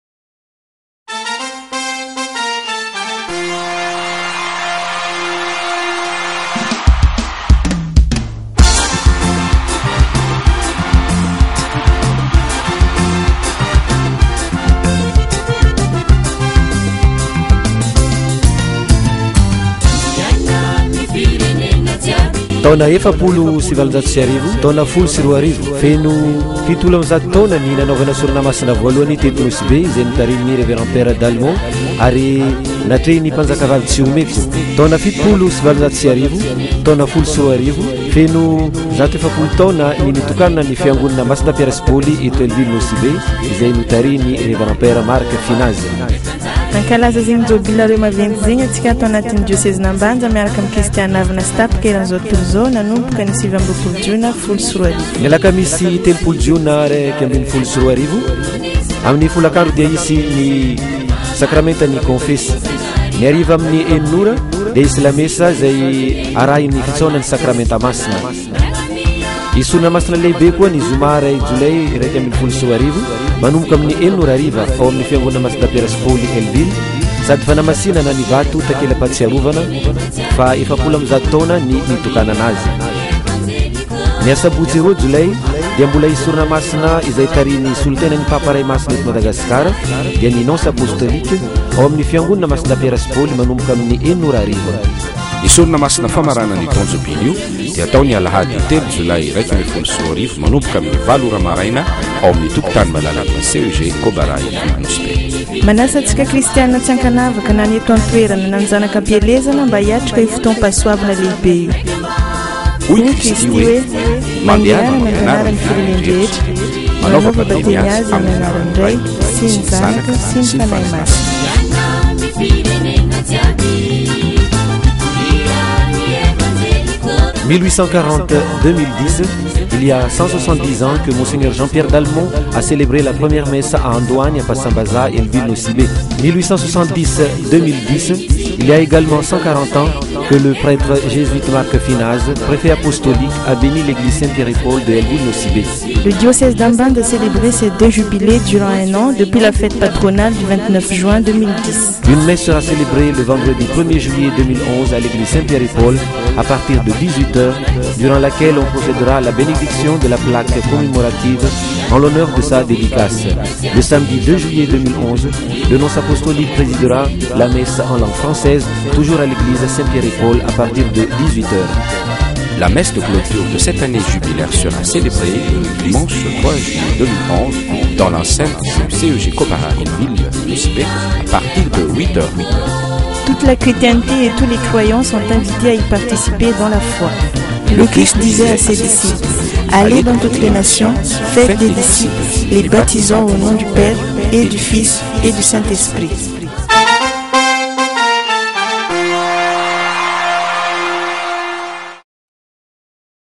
ECAR NOSY-BE CLOTURE JUBILE.FLV
Jubilé 170ème de la première messe à Nosy-Be Hell - Ville.
Jubilé 140ème de la dédicace de l'église SS Pierre et Paul.